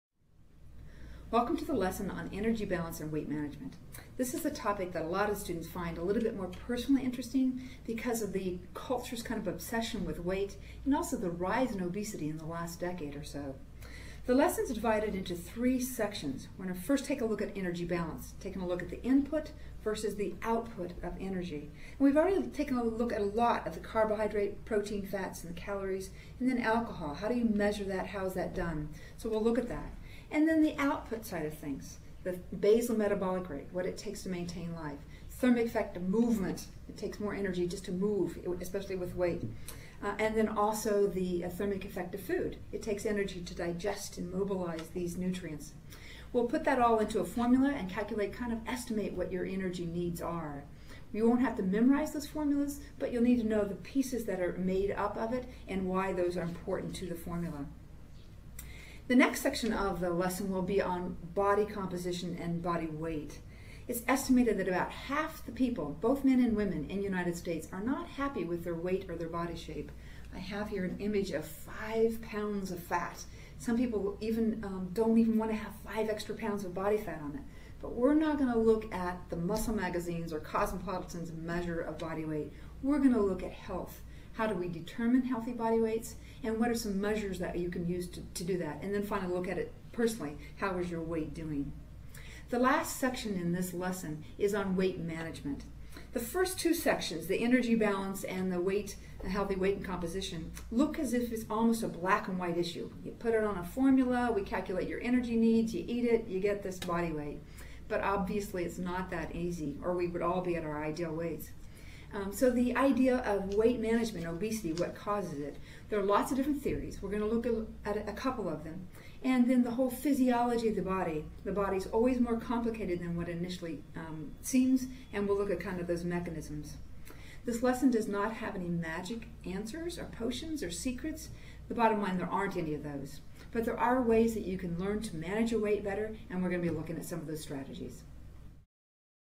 Overview of Weight Management
Overview of weight management lecture for FSHN 167 Introductory Human Nutrition at Iowa State University.